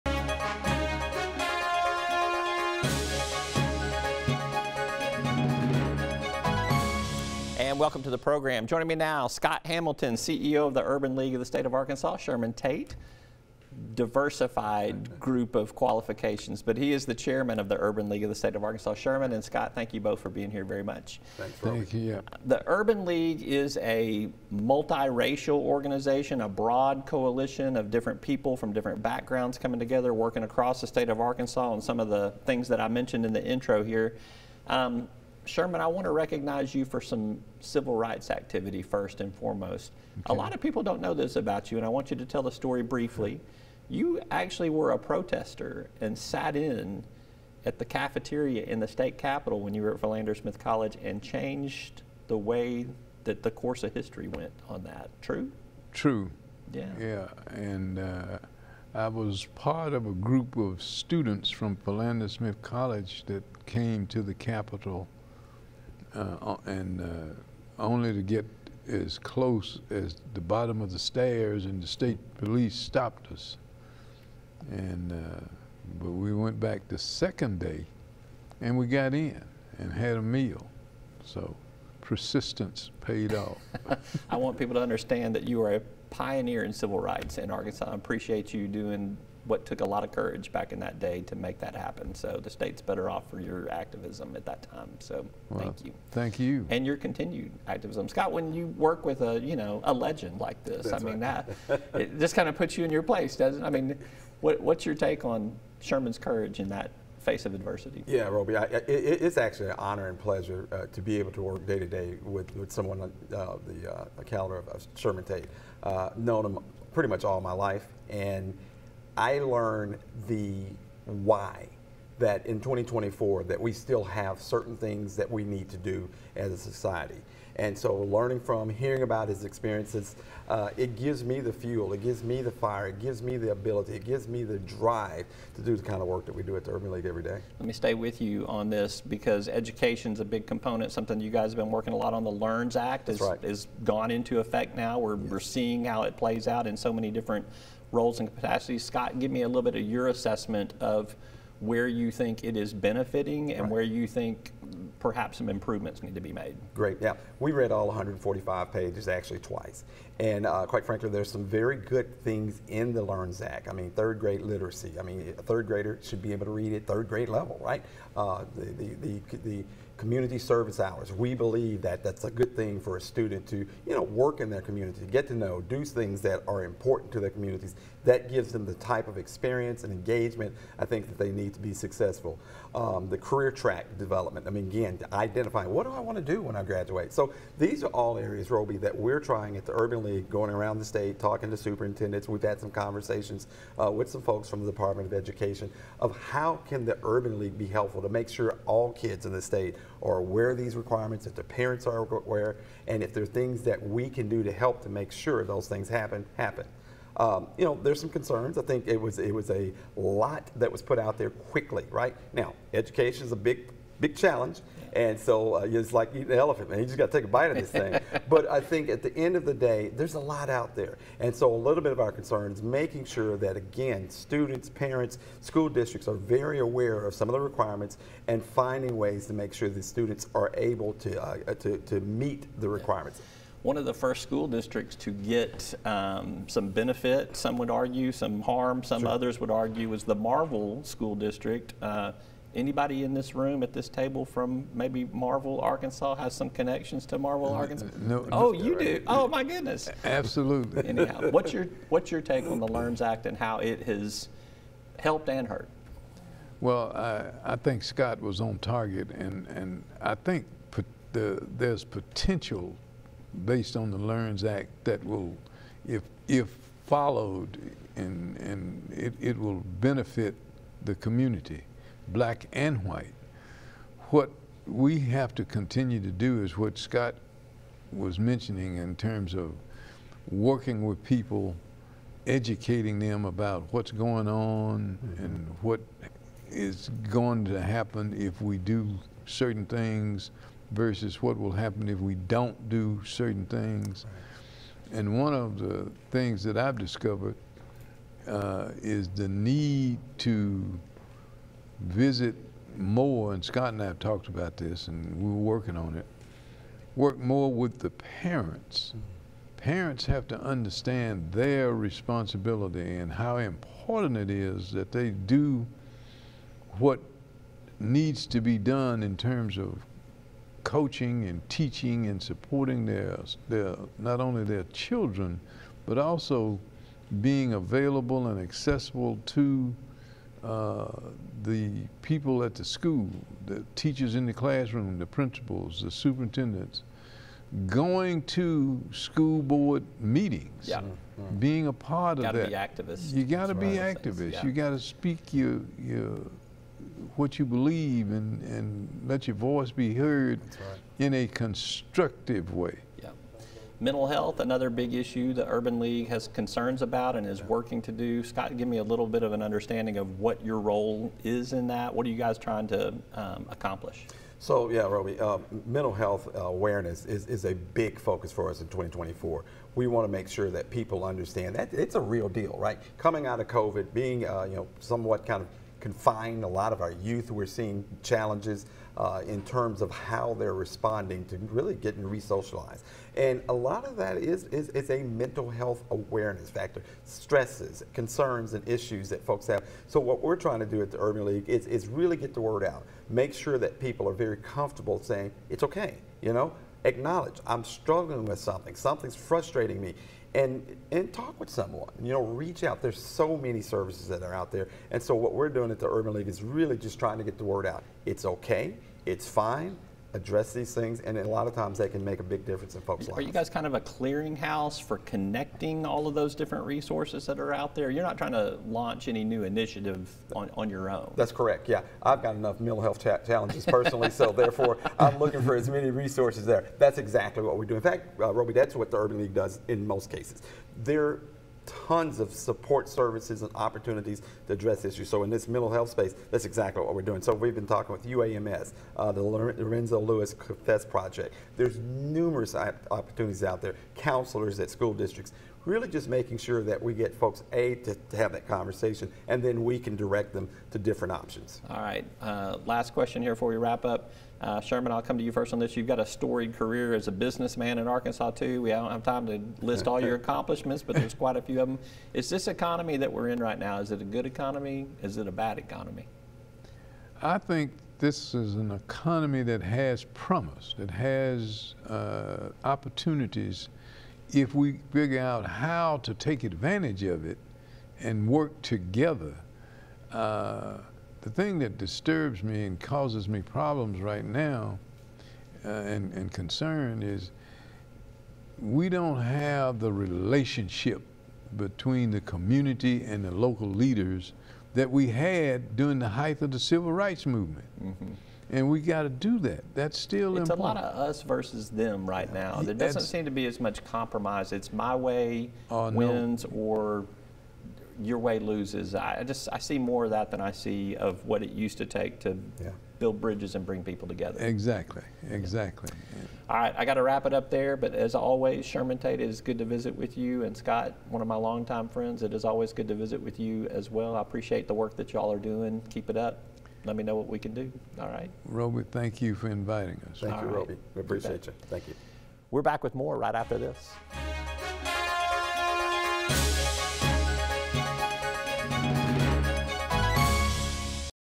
Sherman Tate, Scott Hamilton 2.11.24
Urban League of Arkansas officials Sherman Tate and Scott Hamilton discuss a range of state issues and share some important, but long-forgotten, civil rights history.